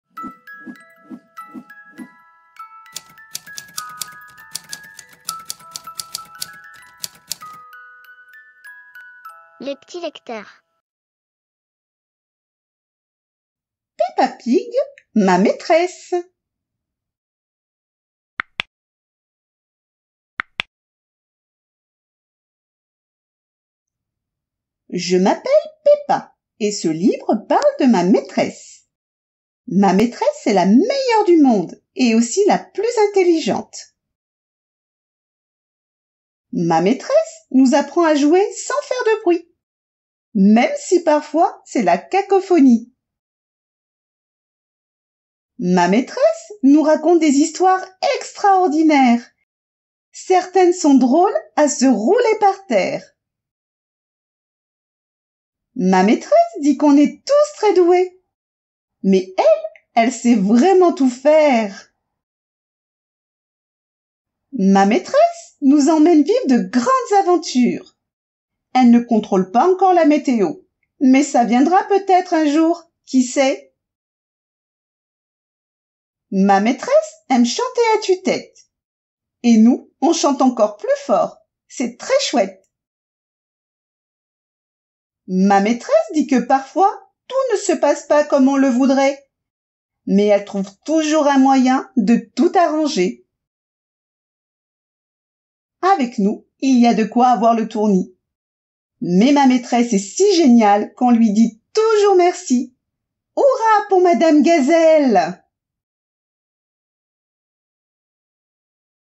Peppa pig Ma maîtresse
Livre audio
Livre audio raconté et monté par les p'tis lecteurs.

Auteurs : Neville Astley et Mark Baker
Éditeur : Hachette Jeunesse

Résumé :
Je m’appelle Peppa et ce livre parle de ma maîtresse. Ma maîtresse est très intelligente. La preuve : elle sait tout faire ! (surtout nous lire des histoires, en jouant un air de rock sur sa guitare.)